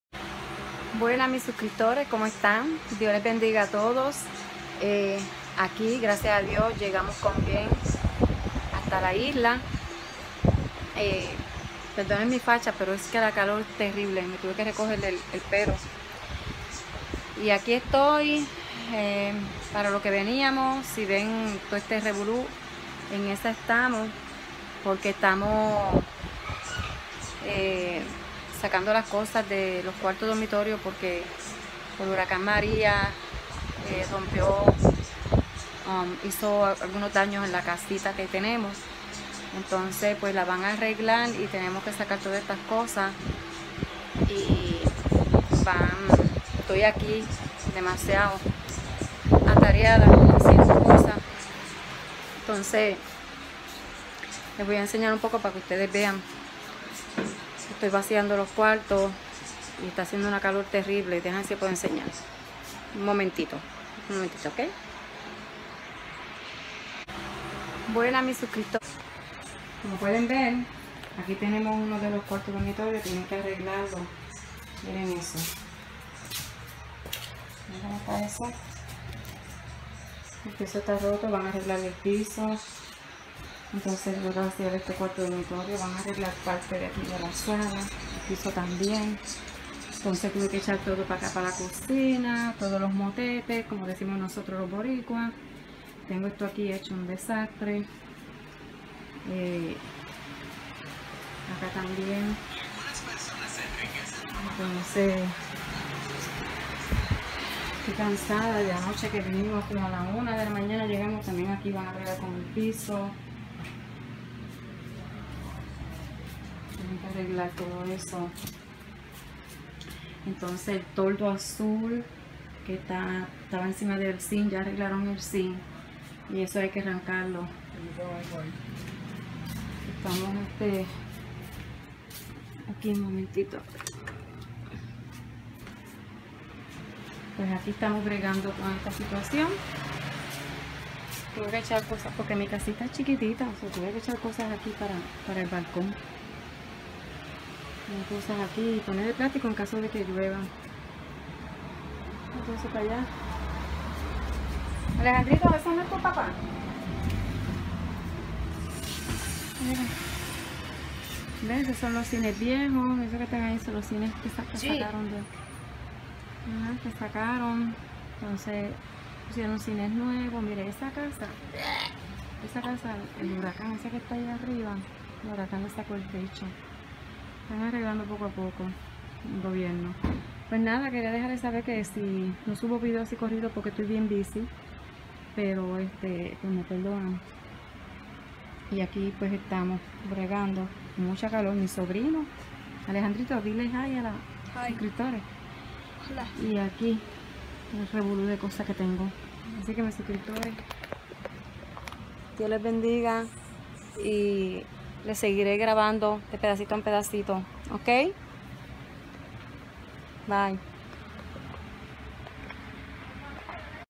Mucho que hacer!
Estoy mudando mis cosas de los dormitori0s porque van a arreglar los pisos , mucho trabajo y calor!